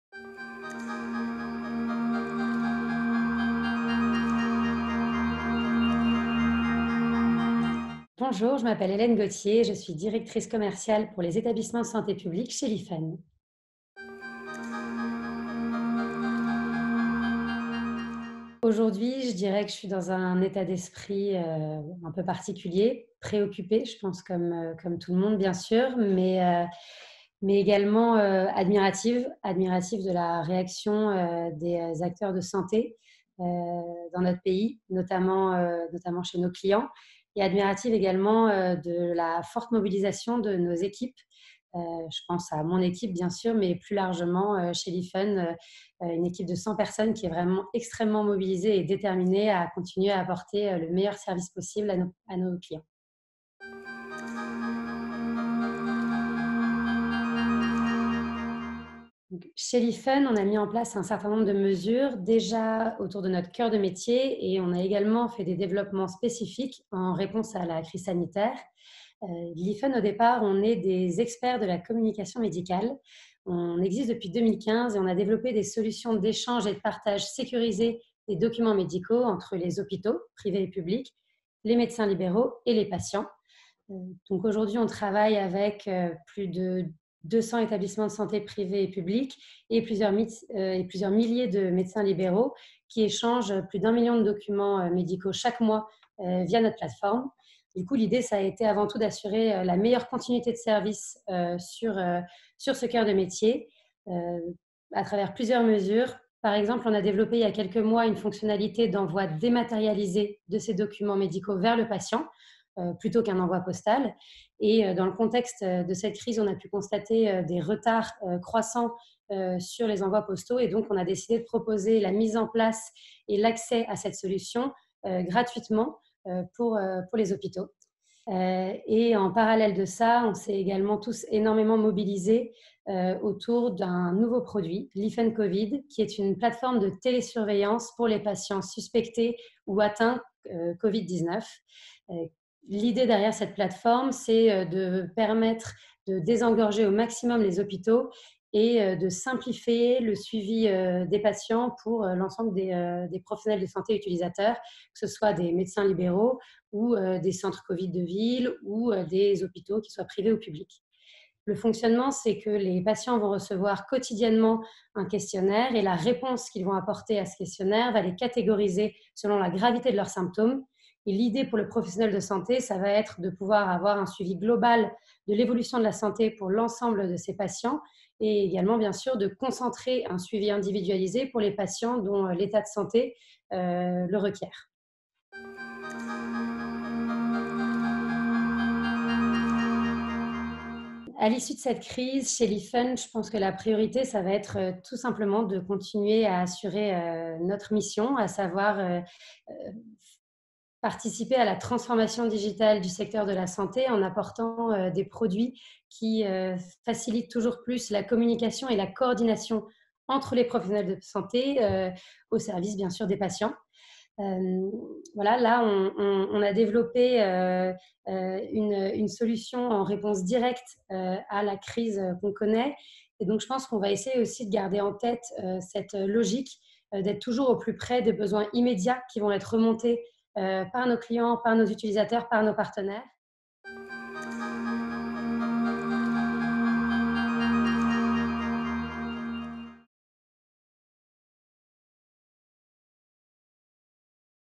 Focus Covid-19 : Zoom sur Hélène Gautier - Lifen
SANTEXPO donne la parole aux entreprises, organisations, sociétés savantes directement impactées et qui œuvrent pour soutenir le système de santé.

Hélène Gautier, Directrice commerciale pour les établissements de santé publique - Lifen

Interviewée le 20 avril, Hélène Gautier revient sur une initiative de Lifen : la création de la plateforme de télésurveillance Lifen Covid pour les patients suspectés ou atteints par le Covid-19.

"L'idée derrière cette plateforme, c'est de permettre de désengorger au maximum les hôpitaux et de simplifier le suivi des patients".

À l'issue de cette crise, la vocation de Lifen sera de continuer de participer à la transformation digitale du secteur de la santé en proposant des solutions pour faciliter la communication et la coordination des professionnels de santé.

Merci Hélène Gautier pour votre témoignage

SANTEXPO, le salon international Santé et Innovation.